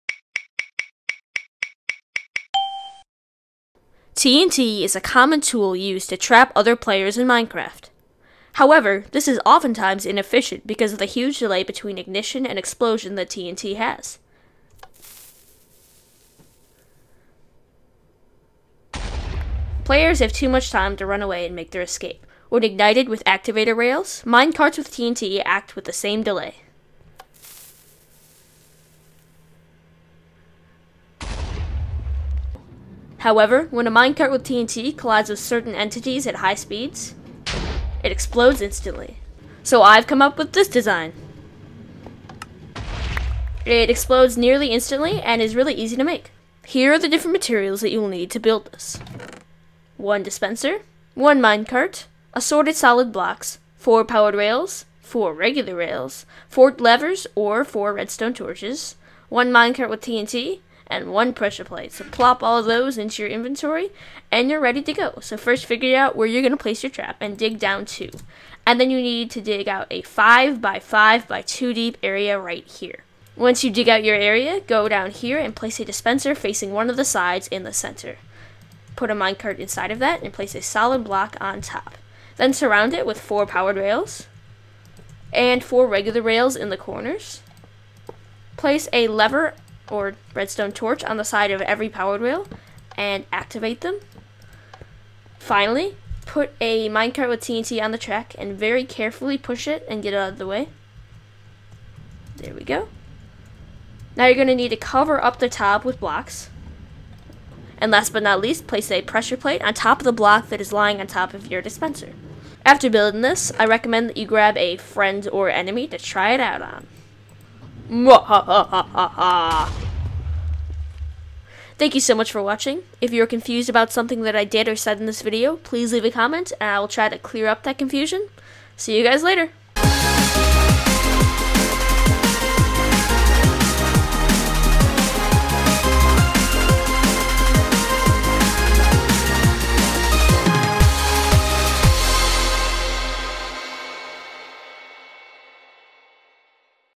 Instant TNT Trap
Hi guys!  I have improved the conventional design for a TNT trap by creating one that activates instantly, making it harder for players to escape before the explosion.